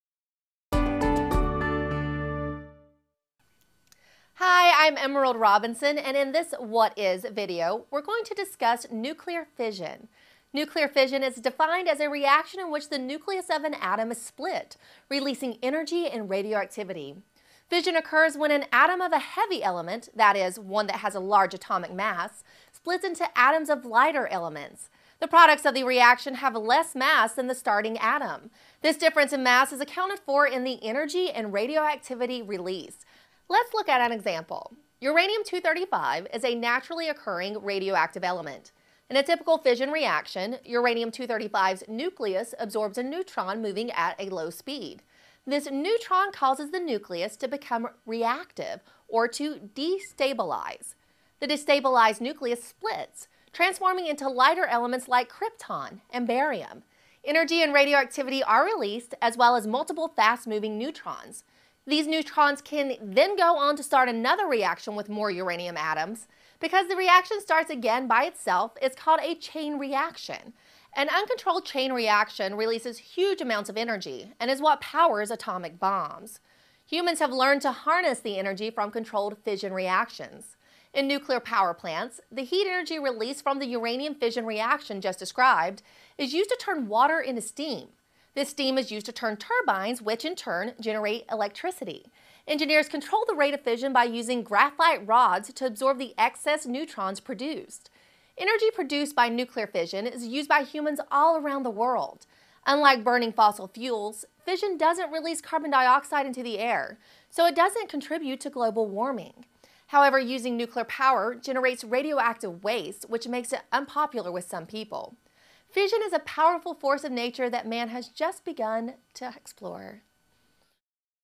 What is Fission?
Hi, I'm Emerald Robinson, and in this "What Is" video, we're going to discuss nuclear fission.

Nuclear fission is defined as a reaction in which the nucleus of an atom is split, releasing energy and radioactivity.

Fission occurs when an atom of a heavy element, that is, one that has a large atomic mass, splits into atoms of lighter elements. The products of the reaction have less mass than the starting atom. This difference in mass is accounted for in the energy and radioactivity released.

Let's look at an example.

Uranium 235 is a naturally occurring radioactive element. In a typical fission reaction, Uranium 235's nucleus absorbs a neutron moving at low speed. This neutron causes the nucleus to become reactive, or to "destabilize."

The destabilized nucleus splits, transforming into lighter elements like krypton and barium. Energy and radioactivity are released, as well as multiple fast-moving neutrons. These neutrons can then go on to start another reaction with more uranium atoms. Because the reaction starts again by itself, it's called a chain reaction. An uncontrolled chain reaction releases huge amounts of energy, and is what powers atomic bombs.

Humans have learned to harness the energy from controlled fission reactions. In nuclear power plants, the heat energy released from the uranium fission reaction just described is used to turn water into steam. This steam is used to turn turbines, which, in turn, generate electricity. Engineers control the rate of fission by using graphite rods to absorb the excess neutrons produced.

Energy produced by nuclear fission is used by humans all around the world. Unlike burning fossil fuels, fission doesn't release carbon dioxide into the air, so it doesn't contribute to global warming. However, using nuclear power generates radioactive waste, which makes it unpopular with some people. Fission is a powerful force of nature that man has just begun/started to explore.